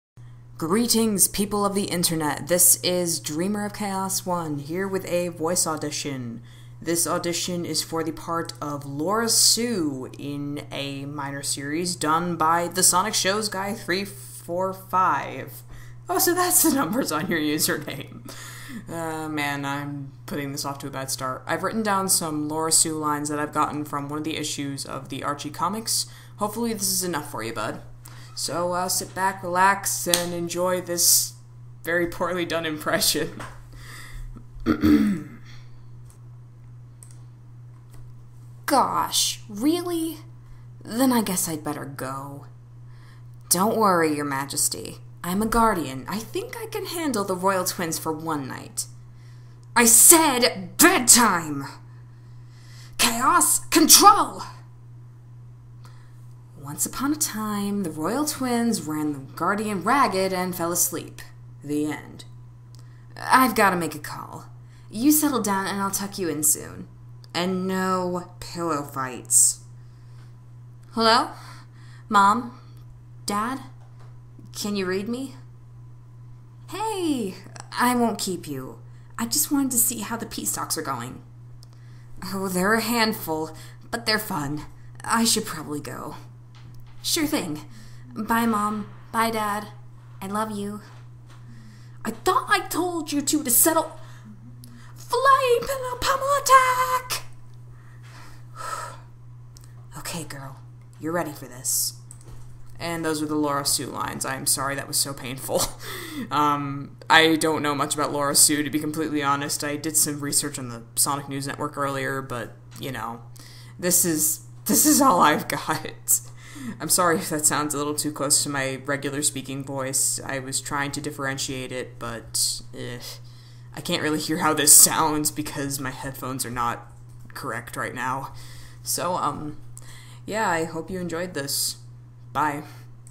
Lara-Su Voice Audition for TheSonicShowsGuy345
Just a voice audition for a character with no official voice actor. XD

Lara-Su, the Future Daughter of Knuckles and Julie-Su, belongs to Archie Comics, same with the images and the lines.

My voice belongs to me.